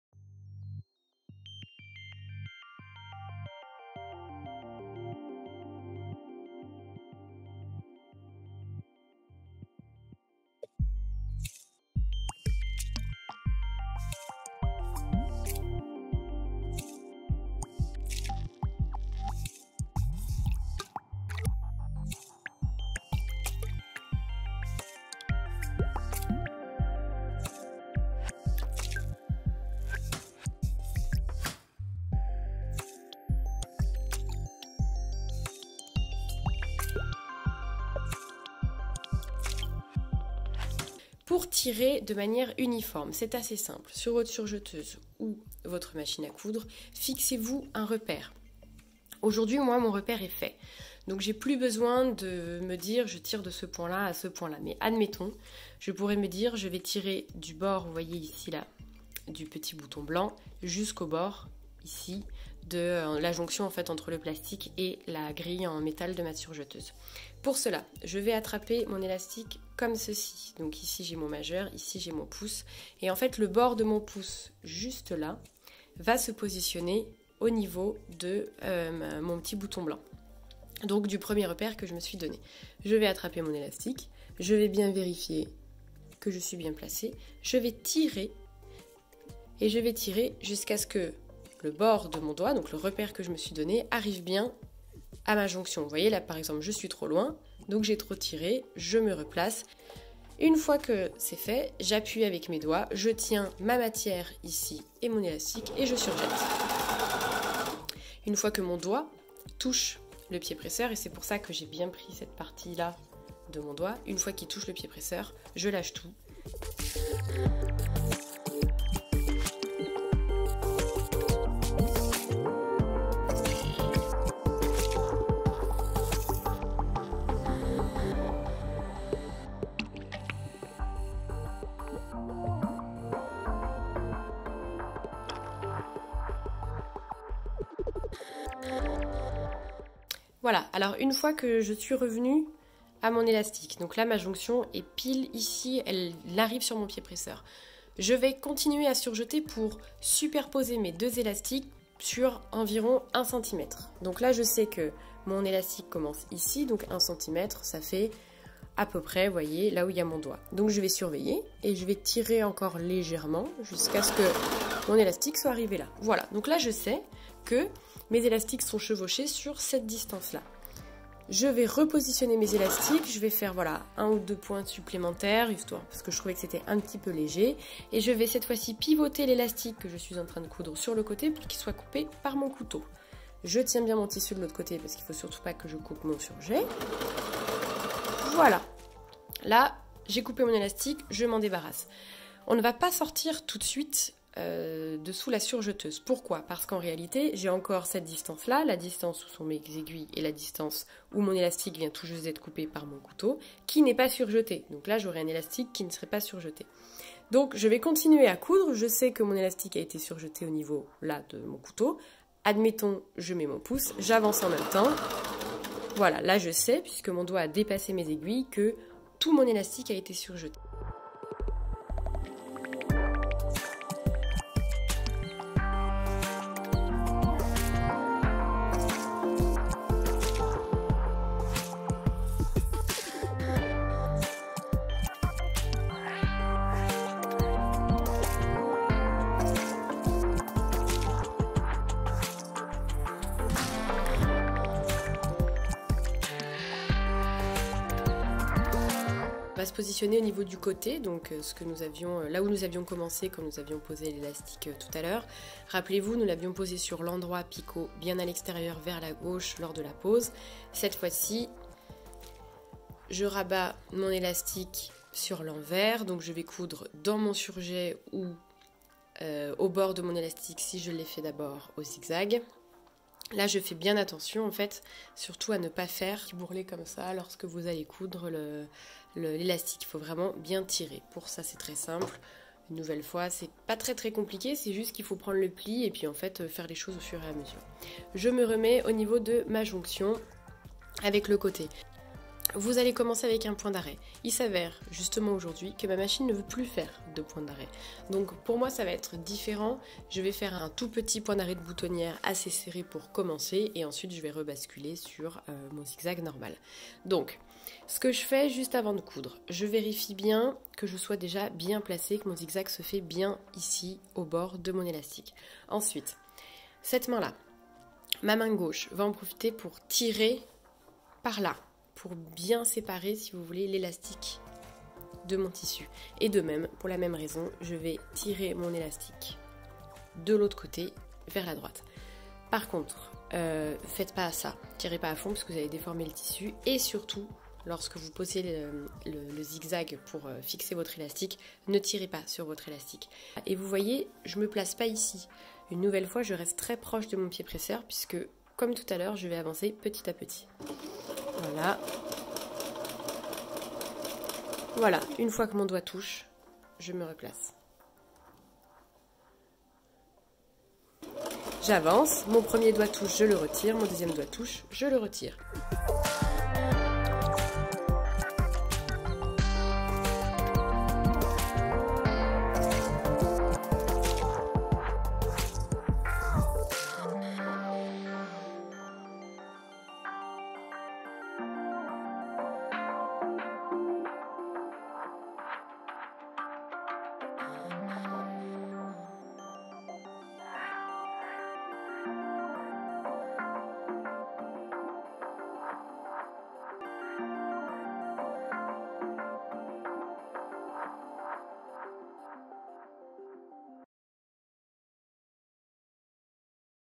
Comment coudre un élastique ?
Comment coudre un élastique sur vos sous-vêtements.
Dans ce tuto je vous livre toutes mes astuces pour une pose d'élastique uniforme.

Pour une pose exclusivement sur MAC, utilisez le zigzag pour la première étape, avec un point d'arrêt au début et à la fin tout en chevauchant votre élastique sur 1 cm.